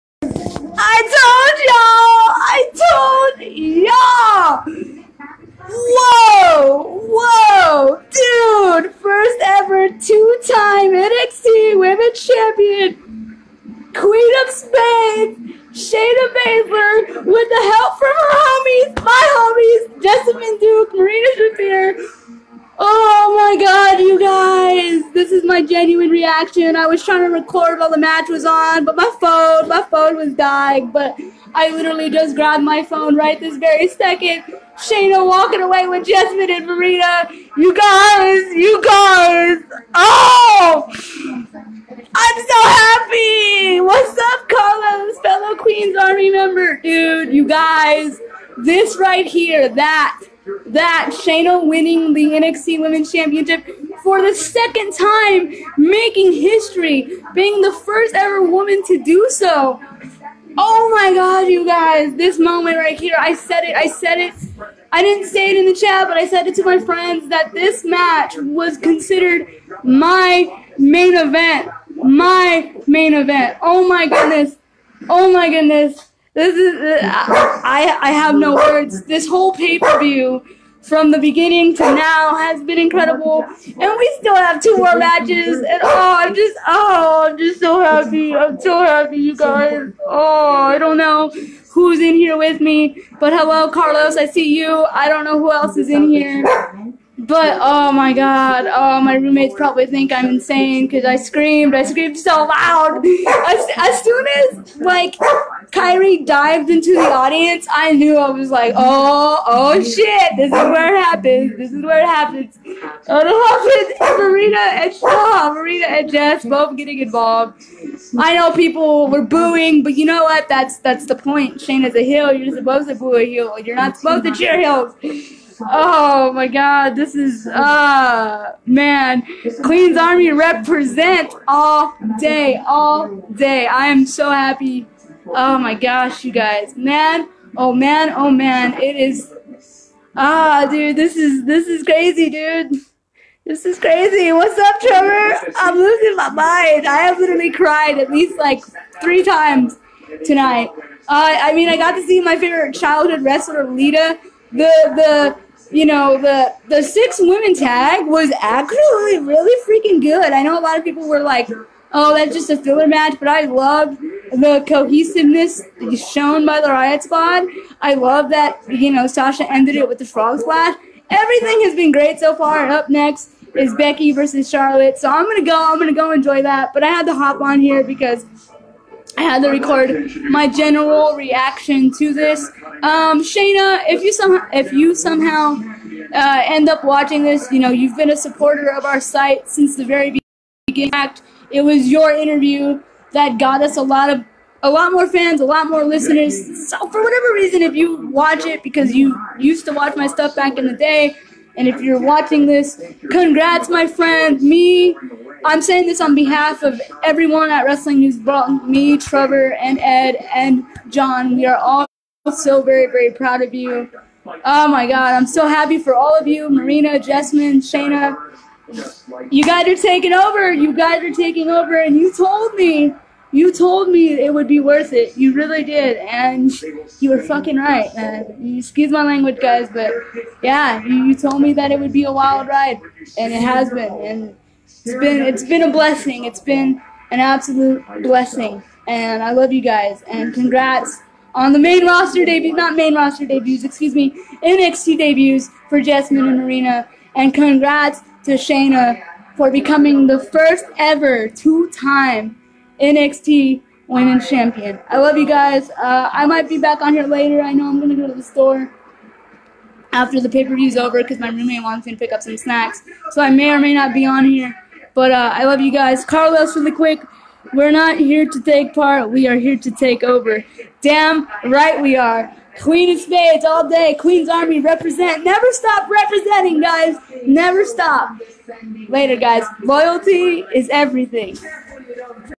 Wrestling News Blog Facebook Live 10 28 18 Vanessa Reacts To 4 Horsewomen @ Evolution
Wrestling News Blog contributor goes live on our Facebook page with instant reactions to the rest of the 4 horsewomen of MMA appearing @ WWE Evolution!